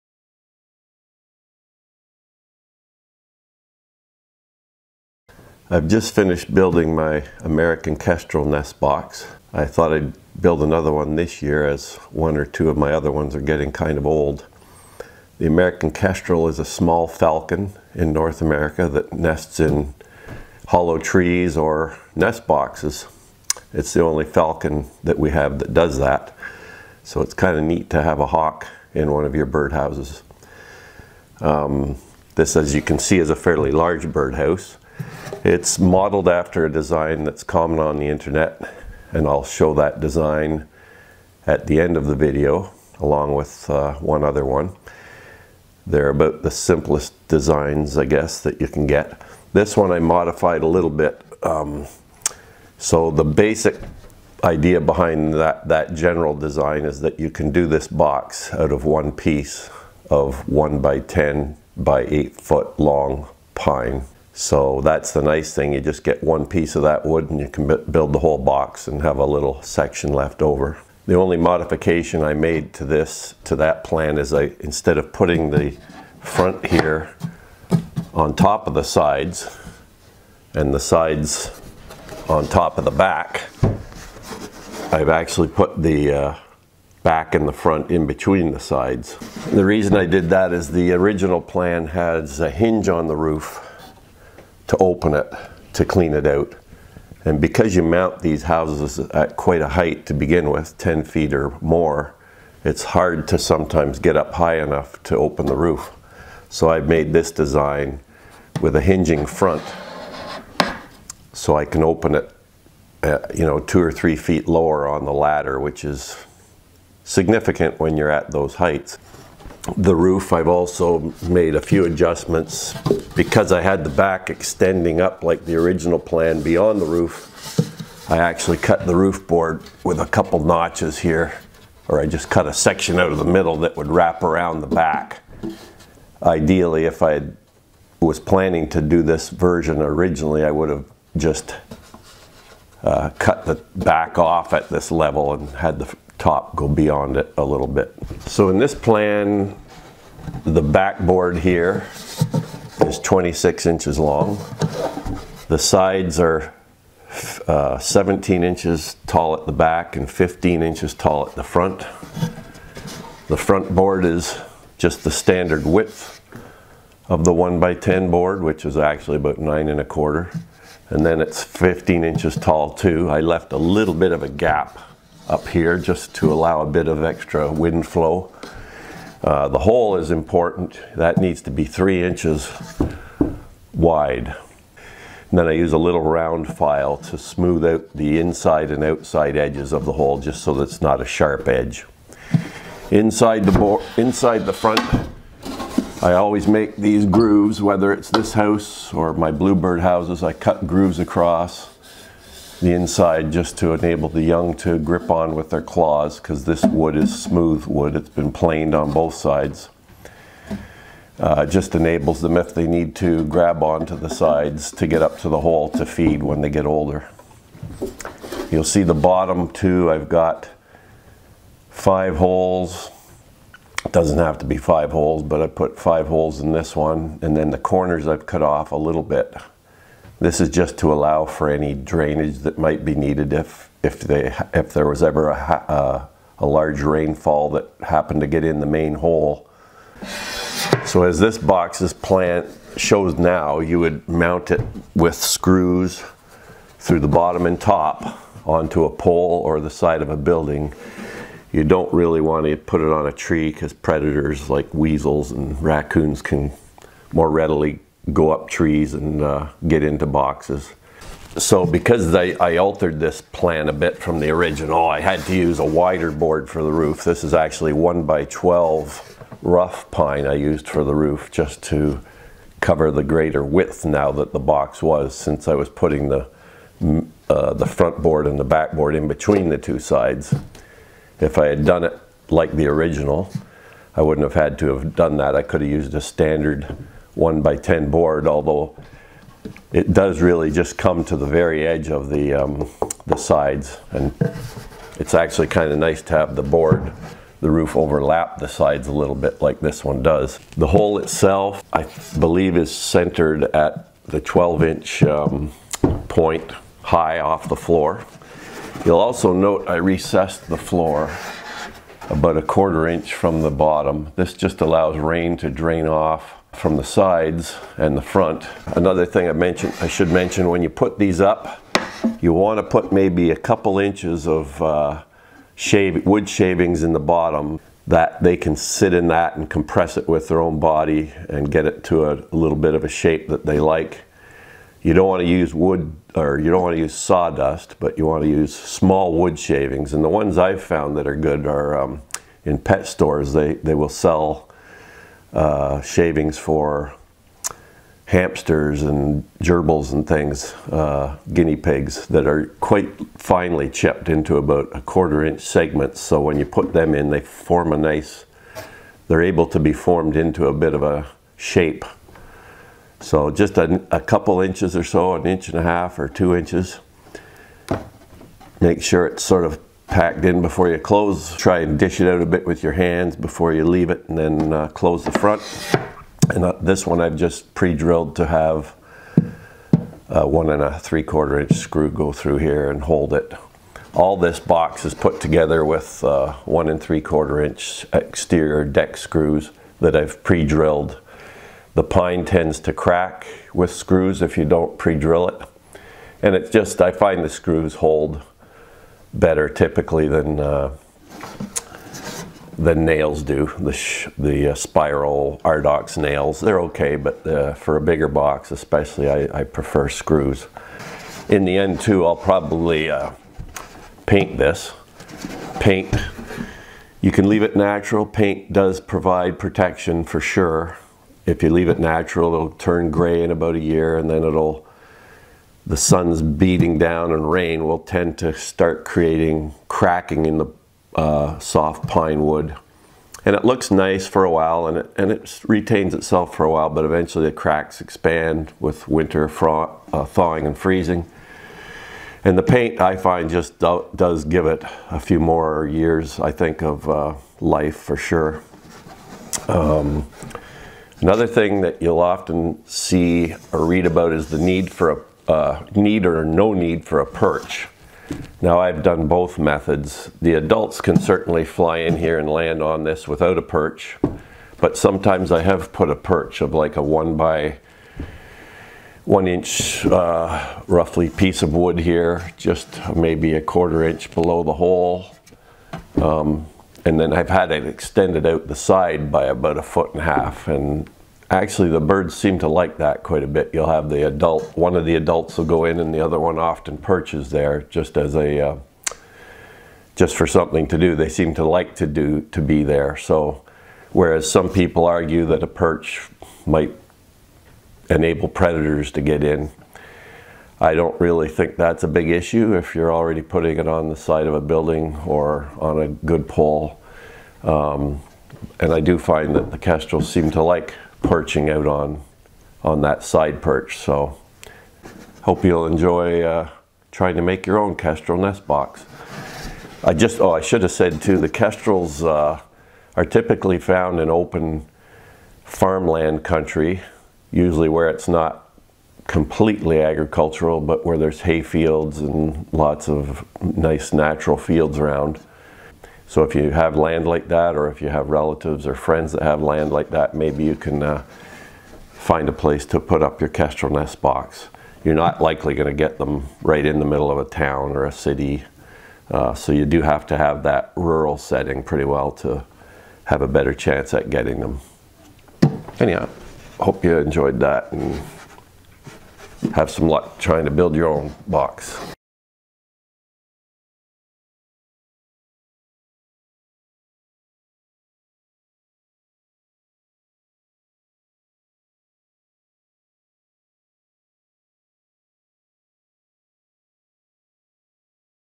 Build an American Kestrel Nest Box - Feb 11, 2021
Hi everyone,

I just finished building a nest box for American Kestrels.  I discuss the general dimensions and design of this type of box, variations in plans and where to place it for the best chance of getting kestrels nesting in it.

At the end I insert a couple of the simpler plans for building this house out of 1"x10"x8" pine or cedar.  Please let me know if you have any specific questions.

Get your tools out and give it a try!